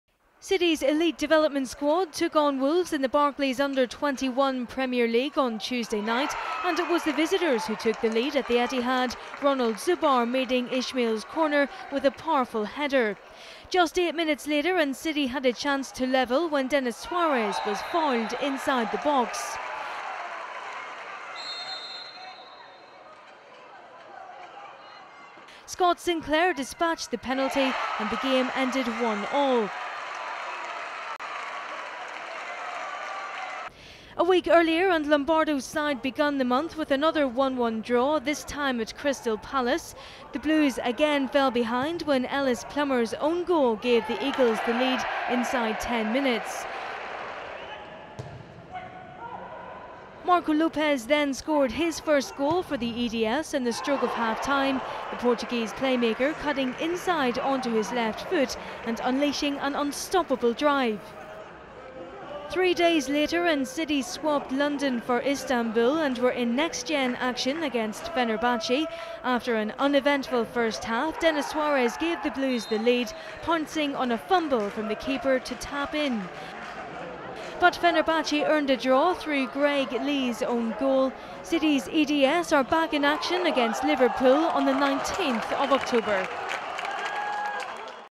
HIGHLIGHTS City EDS v Wolves, Crystal Palace and Fenerbahce
All the goals from City Elite Development Squad's October fixtures so far.

CITY v WOLVES
Scott Sinclair's first goal in a City shirt 10 minutes from time cancels out Ronald Zubar header.

CRYSTAL PALACE v CITY
Marcos Lopes notched his first EDS goal to cancel out Palace's early lead but despite the lion's share of chances, City had to be content with a point on an unseasonably balmy night in South London.

FENERBAHCE v CITY
Denis Suarez gave City the lead shortly after the break but a second freak own goal in the space of a week cost the Blues' maximum points.